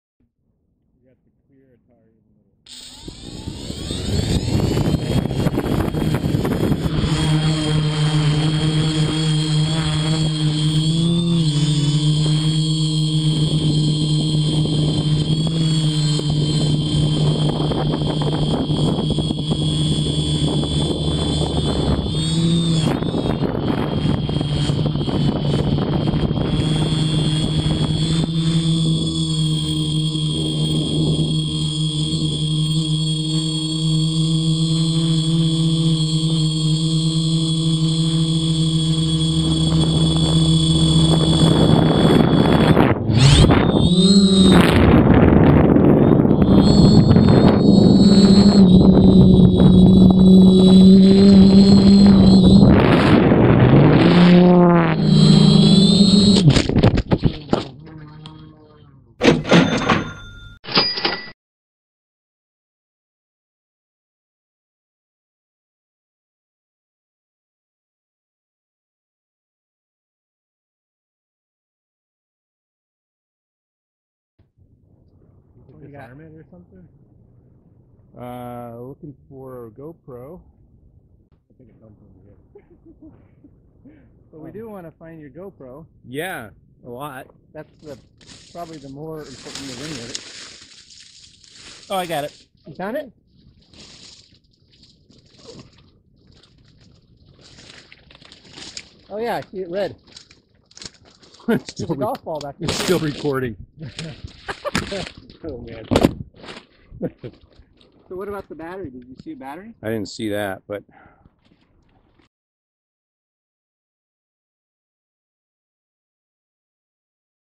Quadcopter Crash Leads to Unfortunate Self Portrait
This was shot with a 3DR-B quad toting a full FPV setup along with my GoPro on a servo based gimbal mount.  Based on the video and resulting damage, it looks like the gimbal took the brunt of the impact.  I still flinch.

I intended to fly to the end of this private runway and run a long side shot in the reverse direction.  As you can see, it never made it that far.

The GoPro was essentially unharmed even though I fly without the protective cover to save weight.

The cause appears to have been a failsafe configuration gone haywire.